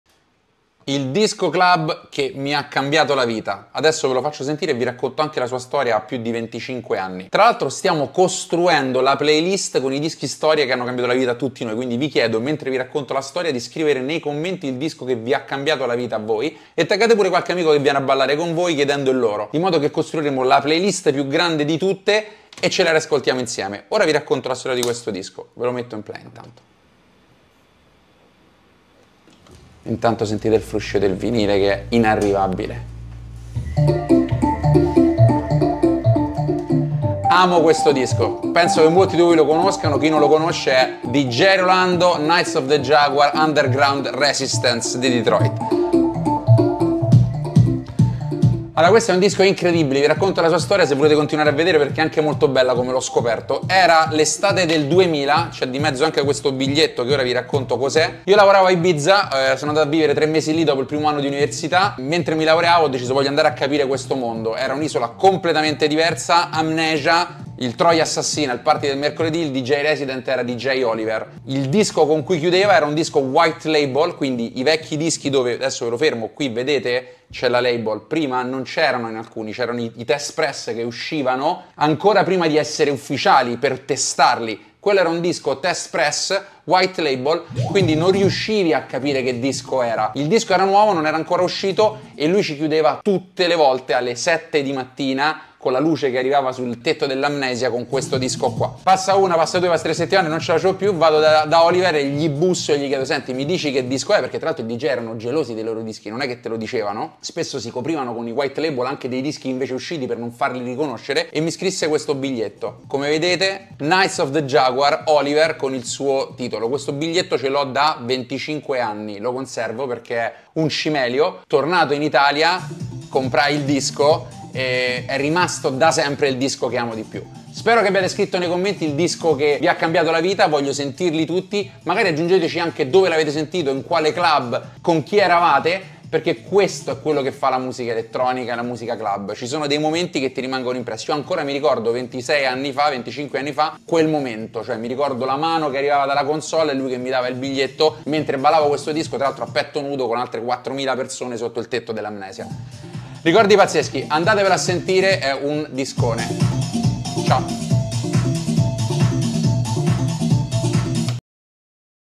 Qual è il Disco Che Vi Ha Cambiato la Vita? | Mat Academy
Facciamo una playlist con i vostri dischi preferiti💽🔥

Raccontatemi nei commenti qual è il disco che vi ha cambiato la vita, il momento in cui l’avete scoperto e taggate l’amico/a con cui l’avete ballato di più🫶🏻

Scopri di più sui nostri corsi, impara dai N1 ed entra nelle etichette che contano grazie al nostro Talent pool CLICCA QUI ➡️